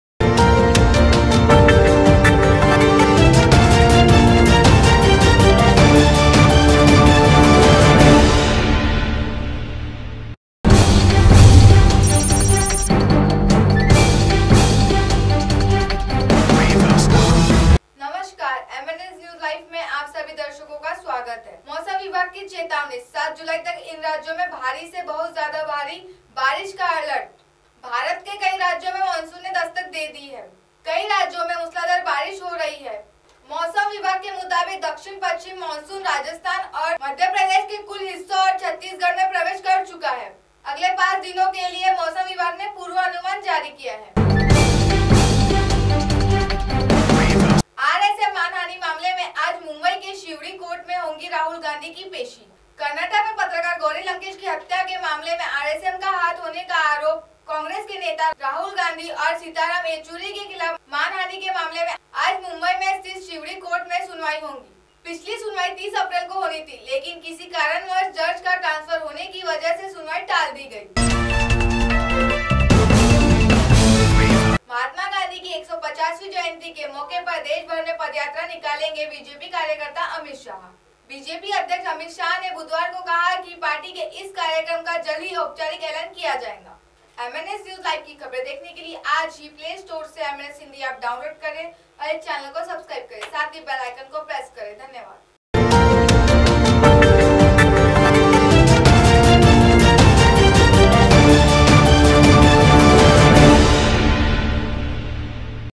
विभाग की चेतावनी ,7 जुलाई तक इन राज्यों में भारी से बहुत ज्यादा बारिश अलर्ट
MNSNEWSLIVE मौसम विभाग की चेतावनी ,7 जुलाई तक इन राज्यों में भारी से बहुत ज्यादा बारिश अलर्ट